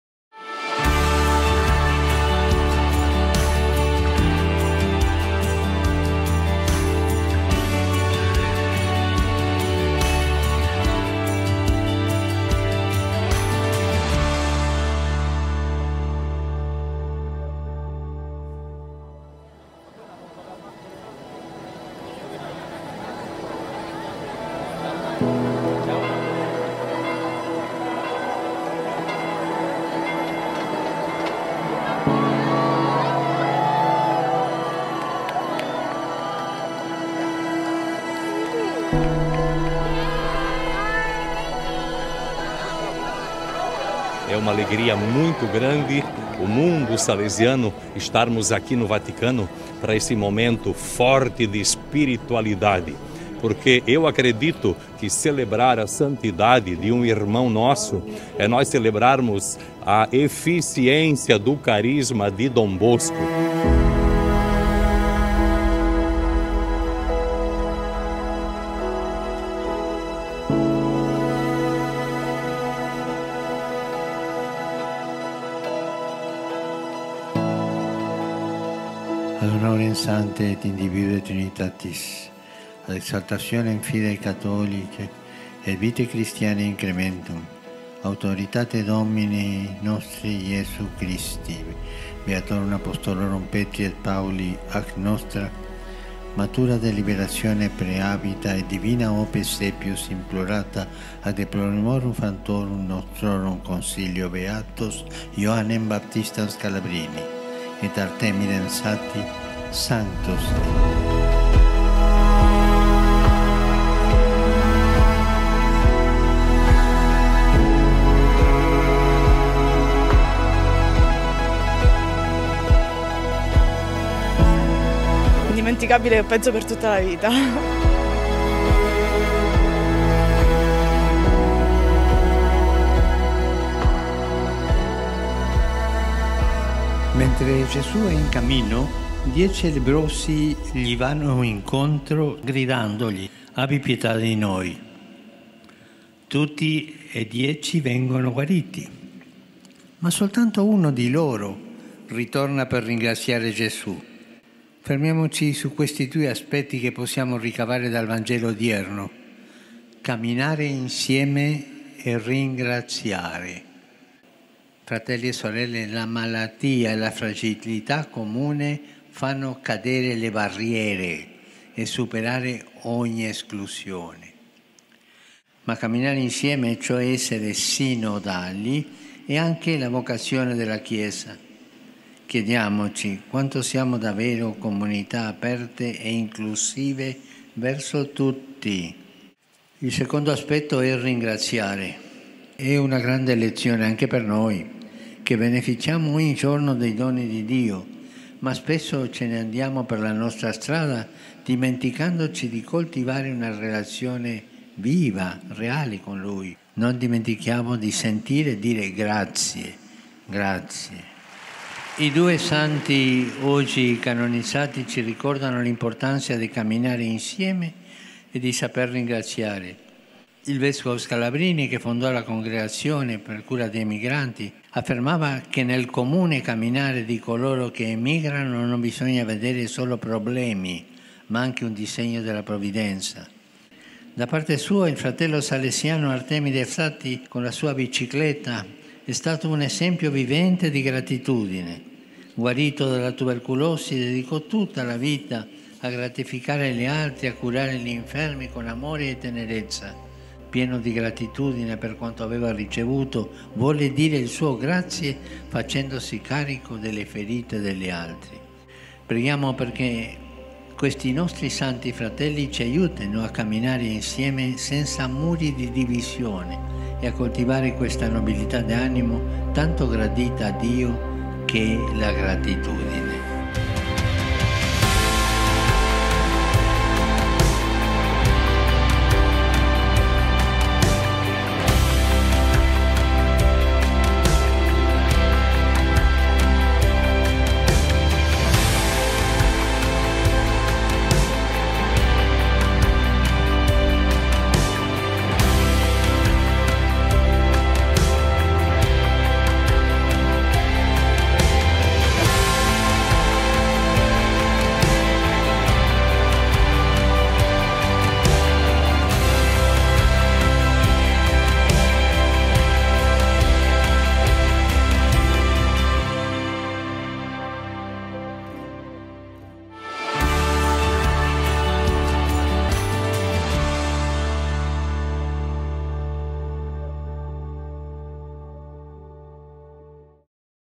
Canonizzazione di Artemide Zatti - Piazza San Pietro
A un mese dalla grande festa per la canonizzazione di Artemide Zatti, abbiamo preparato una sintesi di tutto ciò che si è svolto in Vaticano, l'8 e il 9 ottobre 2022, insieme a Papa Francesco, al Rettor Maggiore e a tutta la Famiglia Salesiana.